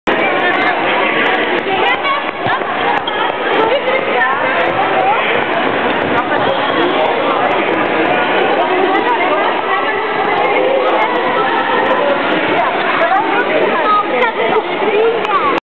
Kijk Ik fiets Lubbeek 2009 2
De sportdienst leert kinderen zonder steunwieltjes fietsen.